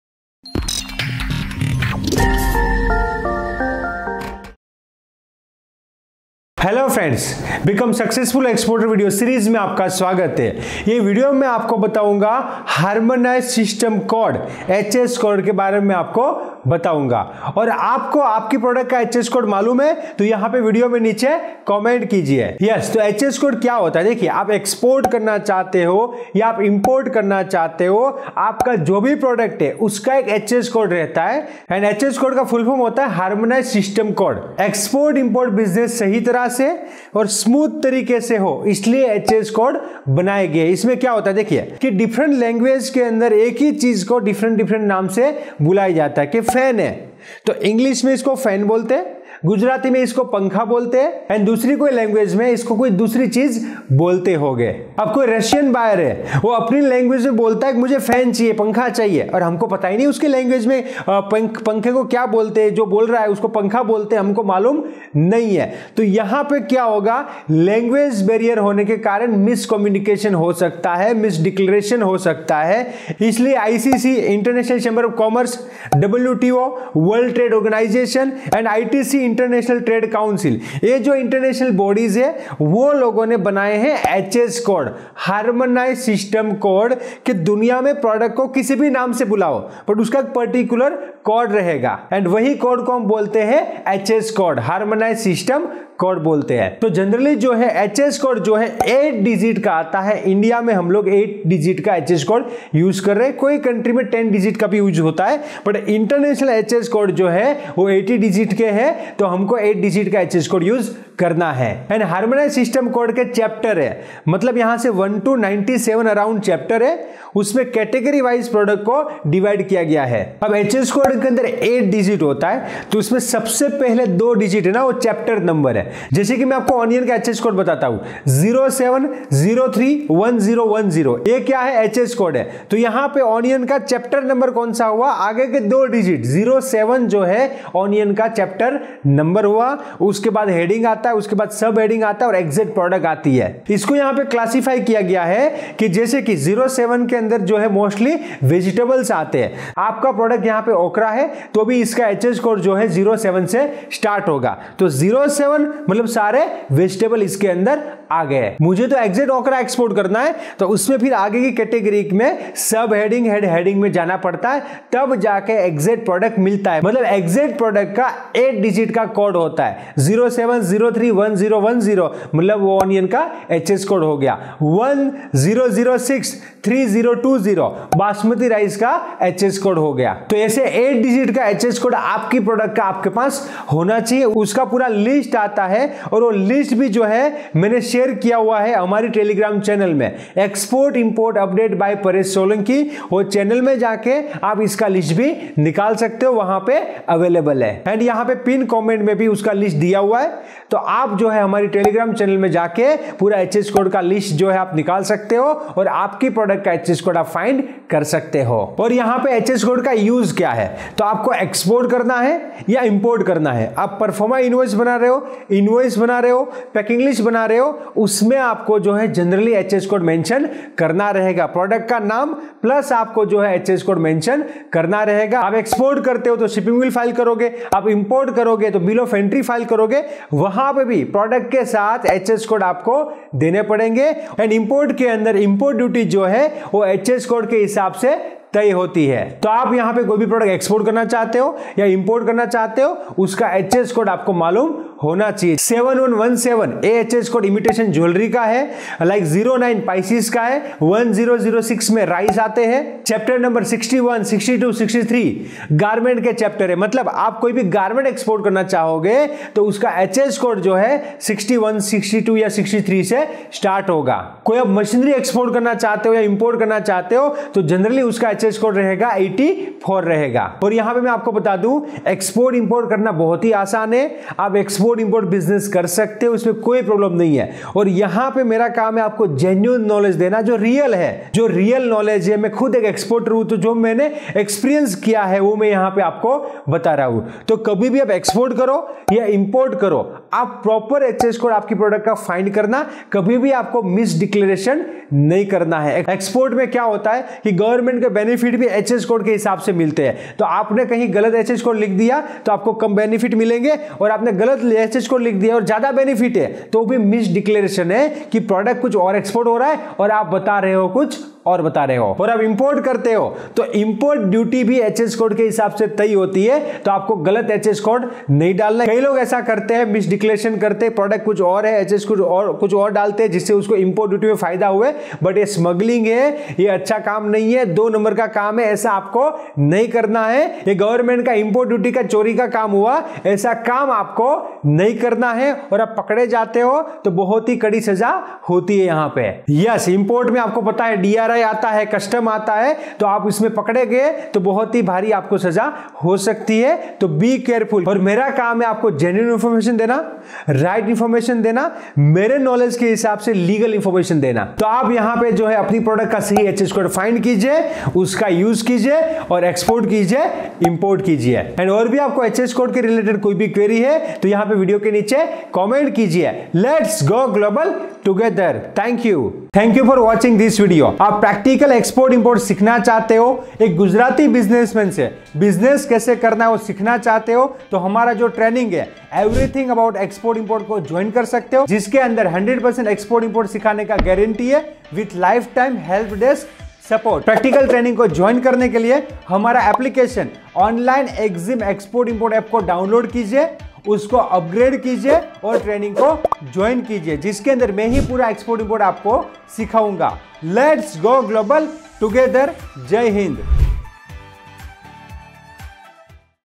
What is HS Code ? | Export Import में Harmonized System Code क्या होता है ? Export Import Business
In this video we are explaining about H S Code in Export Import Business.
H S Code is very important in Export Import Business. Among industry classification systems, Harmonized System (HS) Codes are commonly used throughout the export process for goods. The Harmonized System is a standardized numerical method of classifying traded products. It is used by customs authorities around the world to identify products when assessing duties and taxes and for gathering statistics.

For Latest Export Import Update